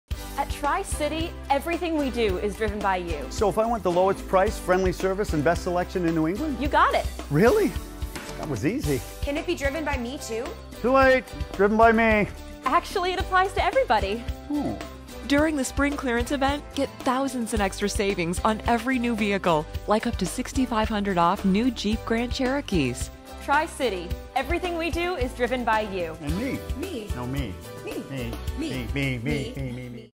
CopywriterJack - Funny Jeep TV Ad
A funny spot I wrote/directed for a Jeep dealership in NH. We also shot the identical spot for Subaru at the same company on the same day. The dealership has had a lot of success with this campaign, as it has really helped to differentiate them in a crowded/competitive marketplace.